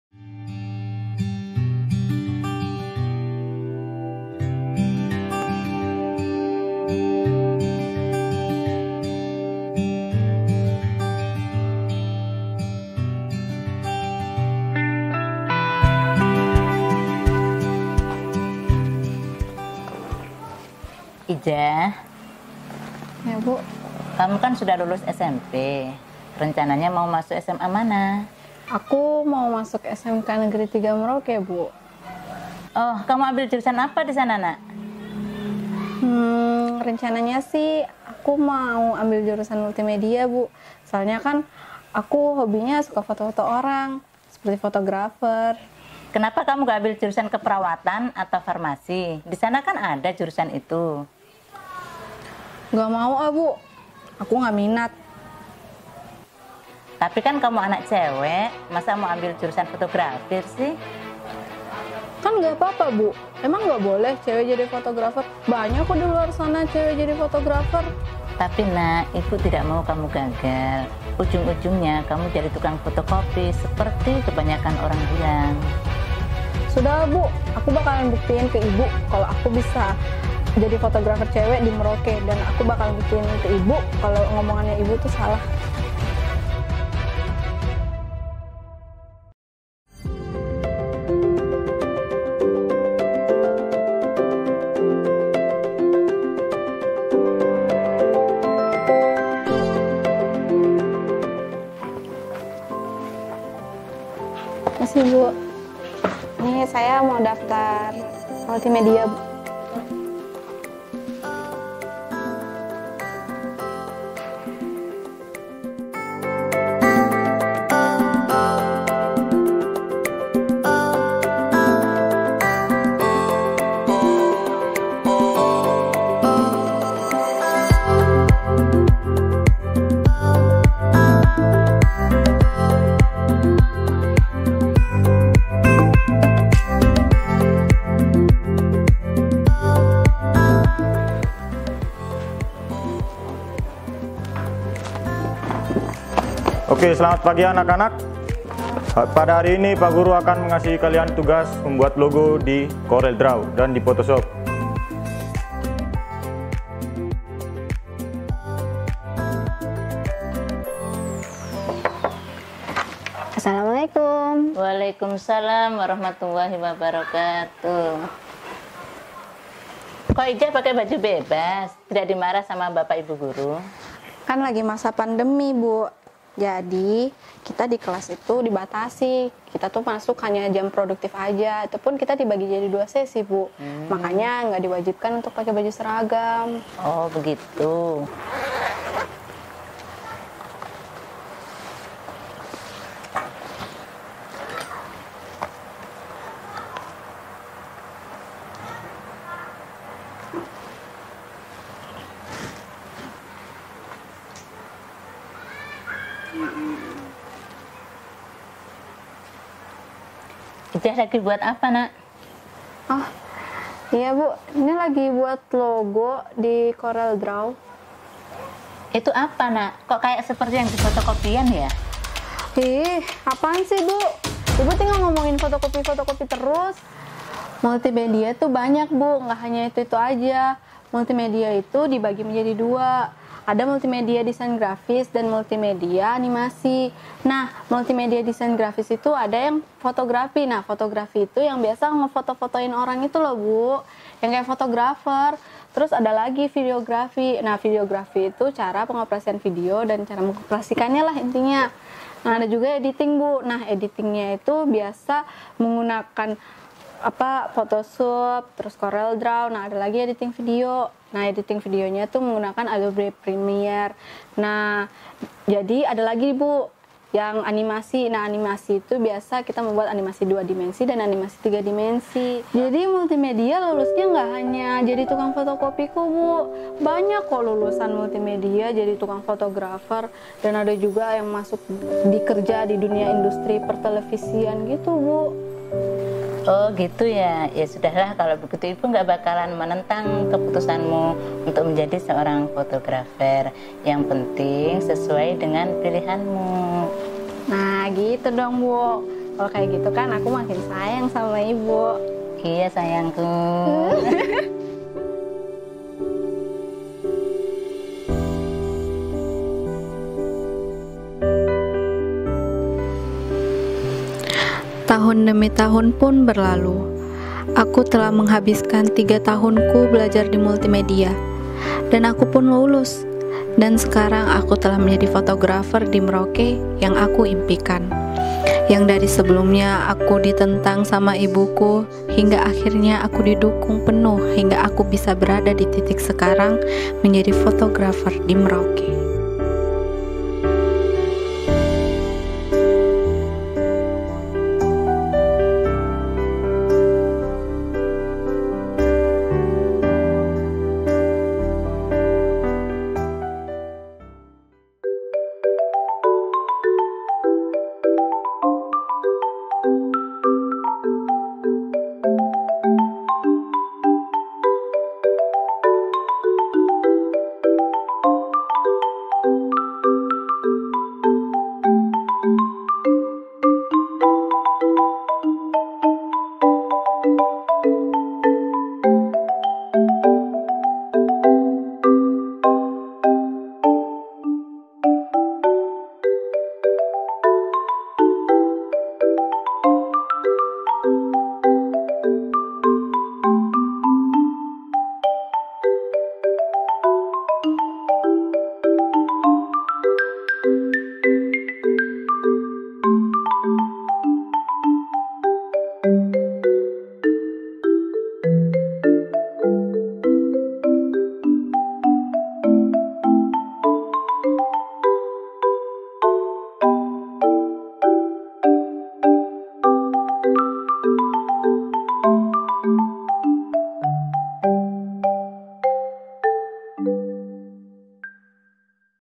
Hobimu Profesimu | Film Pendek Fiksi
Film karya ANDIKA PUTRA SUYIDA kelas XII Jurusan Multimedia. Ujian Kompetensi Keahlian (UKK) Tahun 2021.